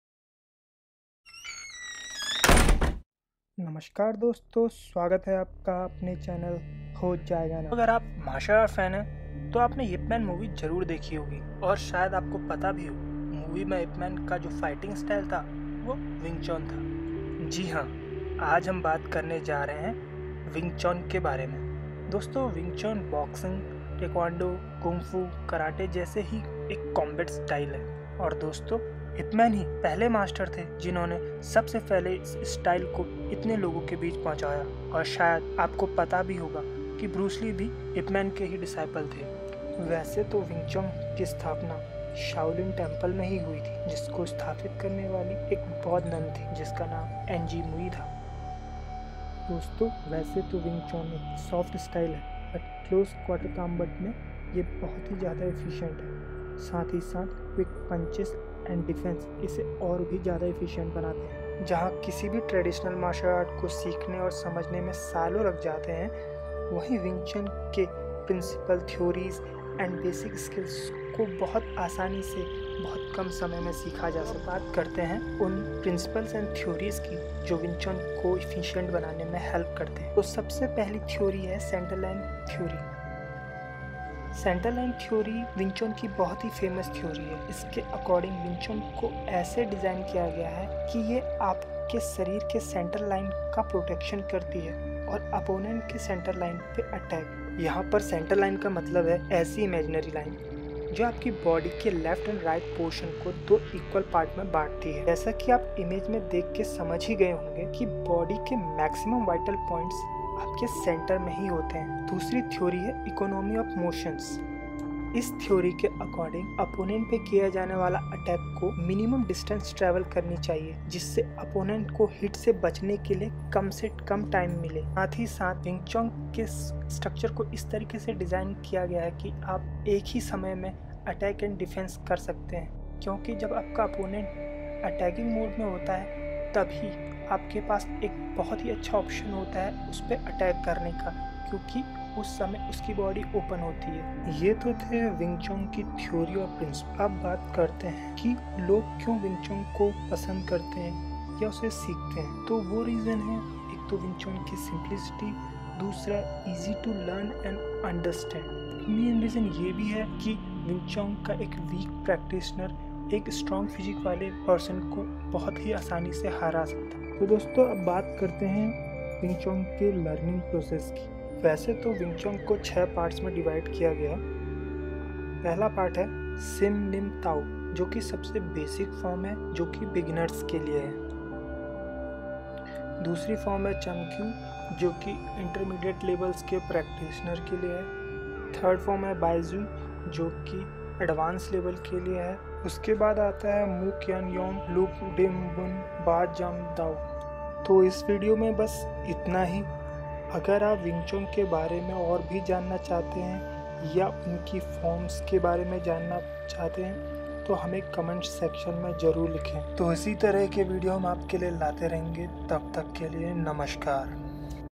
Wing Chun | The art of fight [Hindi]
Wing Chun is a Chinese martial art that is very efficient in close-quarter combat.

Timecode -
0:00 - Introduction
0:47 - Basic Principles of Wing Chun
2:18 - Learning process of Wing Chun